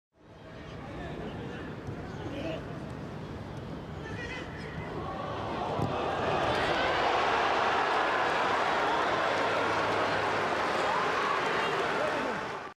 2015 FIFA Goal of the Year - Wendell Lira
This is the amazing goal that won the 2015 FIFA Puskas Award for Goal of the Year.